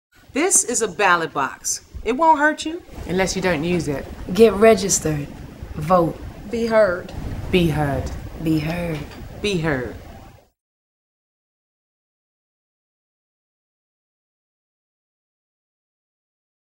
VoteForChange.com: Ballot Box
Queen Latifah, Sophie Okonedo, Alicia Keys, and Jennifer Hudson want you to register to vote, request an absentee ballot, and find your polling location on November 4, 2008.